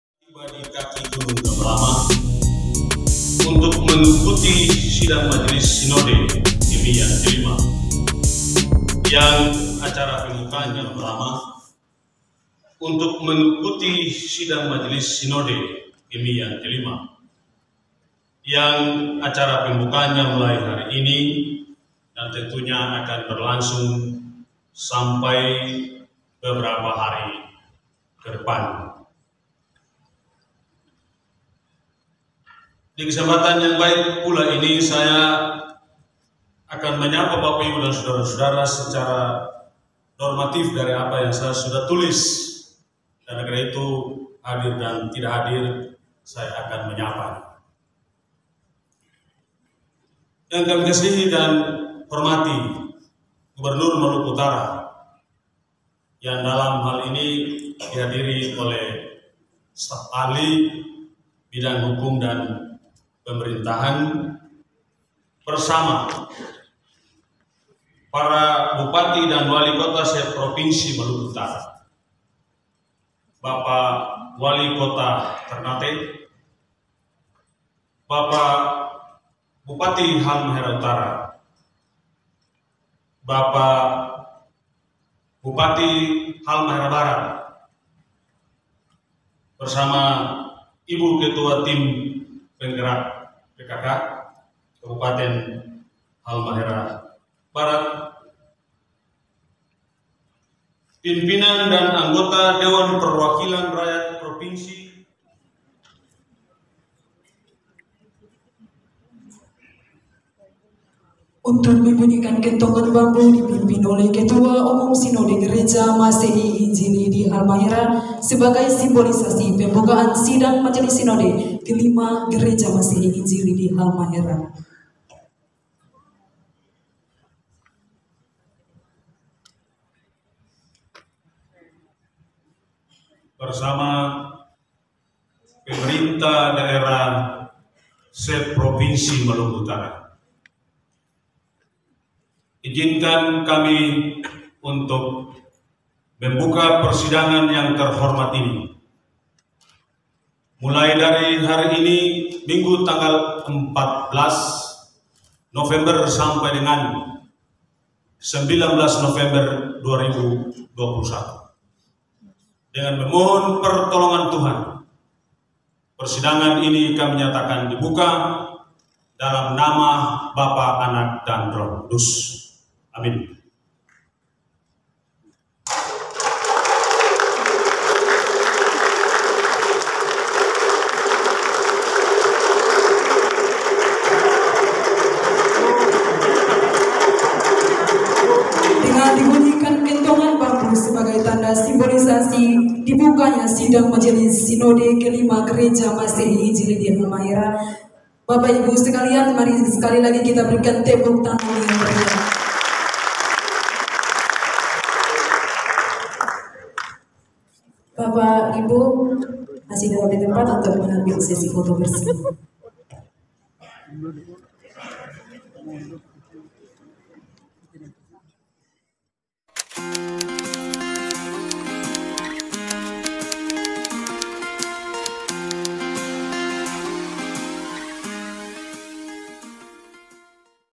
Pembukaan Sidang Majelis Sinode V GMIH #2021
SIDANG MAJELIS SINODE KE-V
Gereja Masehi injili di Halmahera

Tema:
"Aku adalah yang awal dan yang akhir"
Sub Tema:
"Melalui SMS Ke-V GMIH Tahun 2021 kita memperkokoh  teologi dan ajaran GMIH"

UTUH
MANDIRI
MISIONER
Bela Sahid Hotel, 18 November 2022